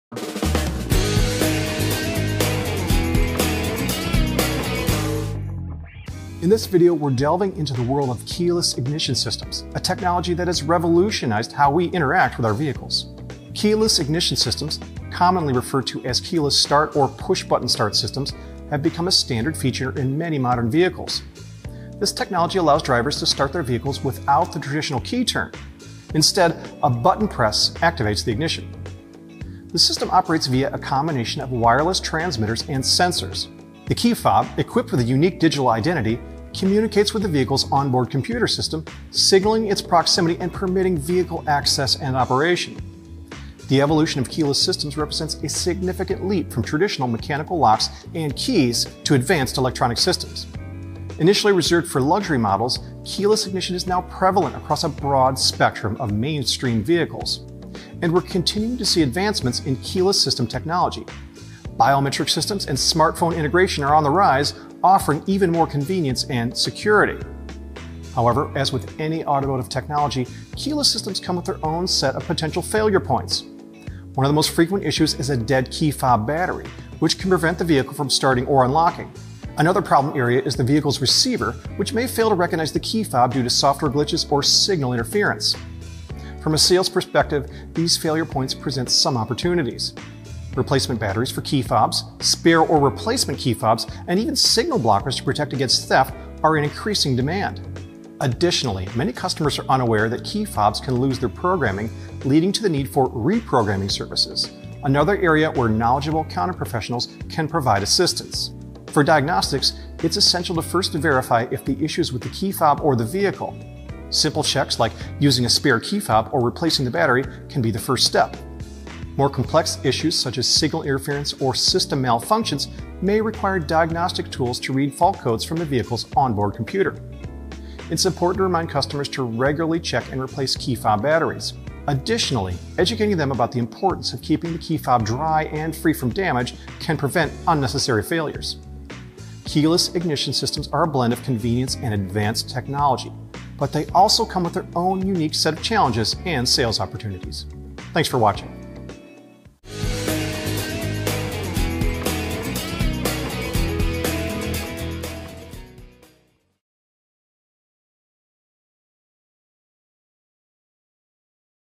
Introduction to Keyless Ignition Systems
Keyless ignition systems, commonly referred to as keyless start or push-button start systems, have become a standard feature in many modern vehicles. This technology allows drivers to start their vehicles without the traditional key turn.  Instead, a button press activates the ignition.